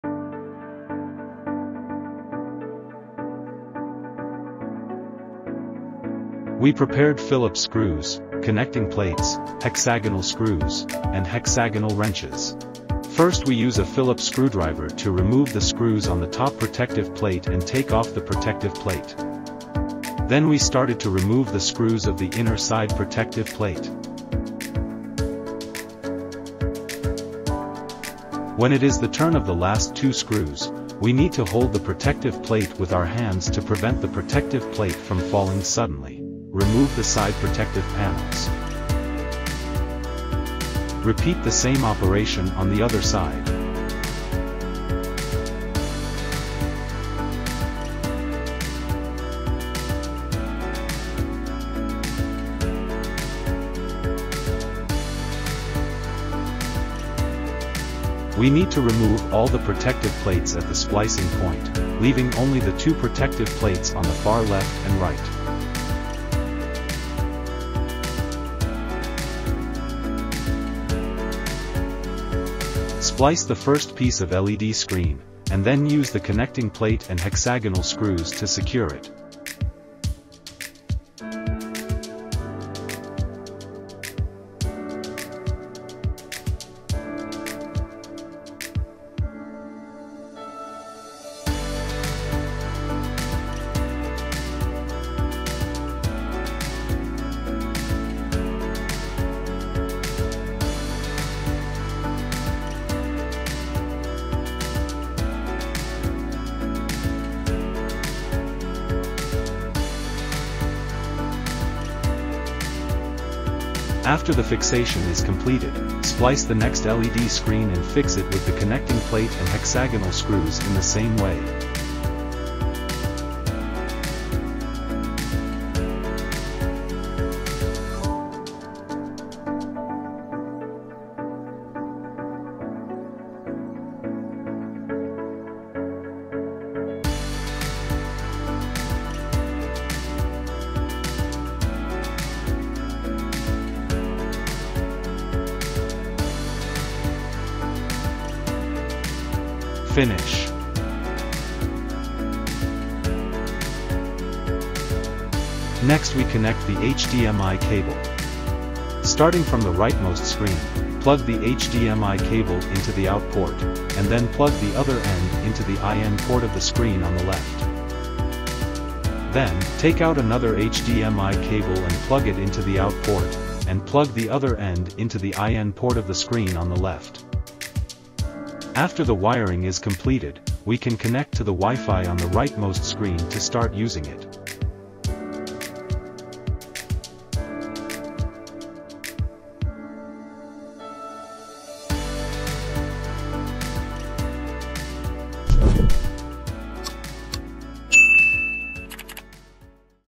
Do You Know How to Splice LED Poster Screen. | EagerLED Led Video Wall Factory
We prepared Phillips screws, connecting plates, hexagonal screws, and hexagonal wrenches.
First we use a Phillips screwdriver to remove the screws on the top protective plate and take off the protective plate. Then we started to remove the screws of the inner side protective plate. When it is the turn of the last two screws, we need to hold the protective plate with our hands to prevent the protective plate from falling suddenly. Remove the side protective panels. Repeat the same operation on the other side. We need to remove all the protective plates at the splicing point, leaving only the two protective plates on the far left and right.
Splice the first piece of LED screen, and then use the connecting plate and hexagonal screws to secure it. After the fixation is completed, splice the next LED screen and fix it with the connecting plate and hexagonal screws in the same way. Finish!
Next we connect the HDMI cable. Starting from the rightmost screen, plug the HDMI cable into the OUT port, and then plug the other end into the IN port of the screen on the left. Then, take out another HDMI cable and plug it into the OUT port, and plug the other end into the IN port of the screen on the left.
After the wiring is completed, we can connect to the WIFI on the rightmost screen to start using it.

// EAGERLED VIDEOS

// ABOUT EAGERLED

// CONTACT EAGERLED